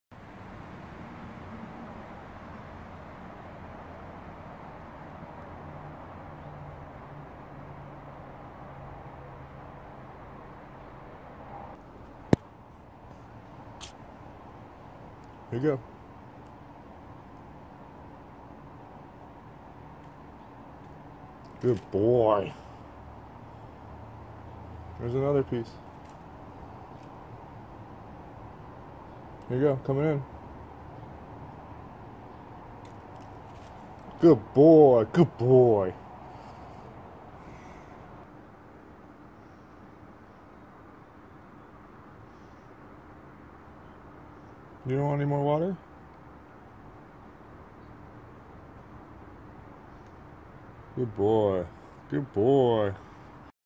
Hand Feeding Baby Crow
A baby crow fell into my yard and I cared for him for a few days. I named him Shawshank. When he was strong enough, he flew back up to the palm tree with his family. It was awesome hanging out with a crow for a few days. He was remarkably smart and seemed like his brain grew with every bite of food. I would feed him fish, ham, and bread all by hand. I was simply motioning from above. He was also extremely thirsty and seemed more in need of water than food from the moment he fell. I would put water on a spoon and he drank it every time. He drank a full water bottle two days in a row!!! Now he lives back with his family and caws at me all the time.